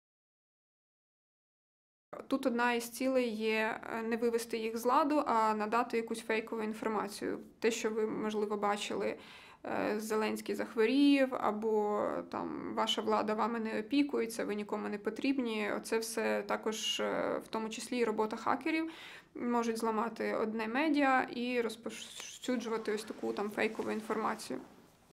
Чому ворожі хакери постійно атакують українські медіа?
російські хакери не припиняють атакувати українські медіа.

Яку мету переслідують кіберзловмисники? Відповідає Євгенія Наконечна, керівниця Урядової команди реагування на комп'ютерні надзвичайні події України CERT-UA, яка діє при Держспецзв’язку.